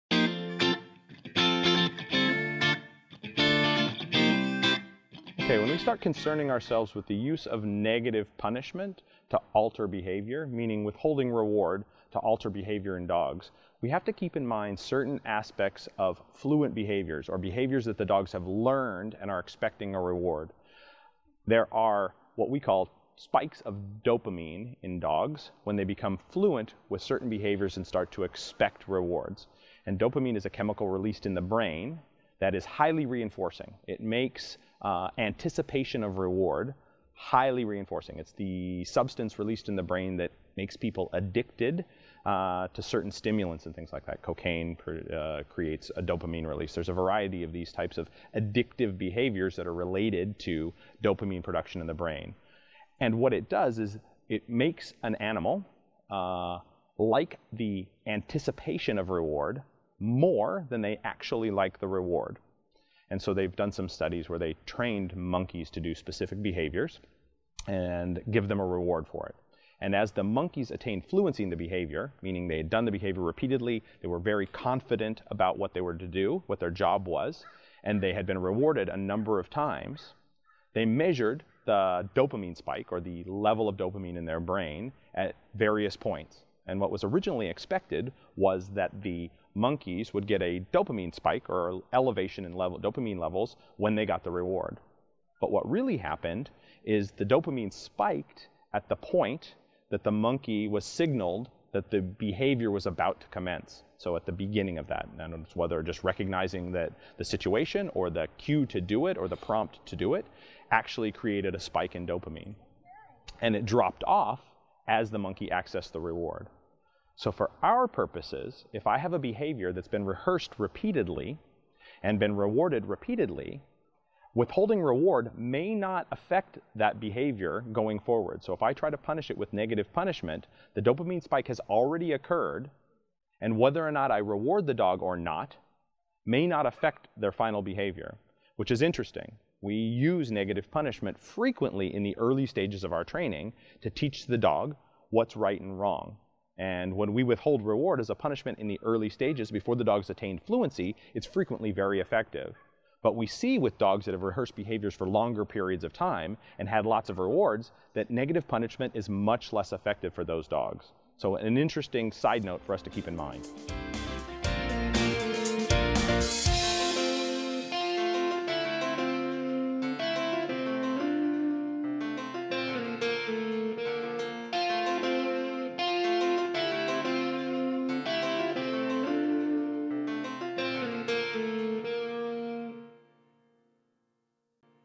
Michael Ellis on Withholding Rewards to Alter Behavior
In this clip, Michael Ellis explains how dopamine and withholding rewards can affect behavior. This is a concept that is important to be aware of as you advance training and your dog becomes fluent in a certain behavior as it can make negative punishment less effective. This clip came from our online course Finishing Work with Michael Ellis.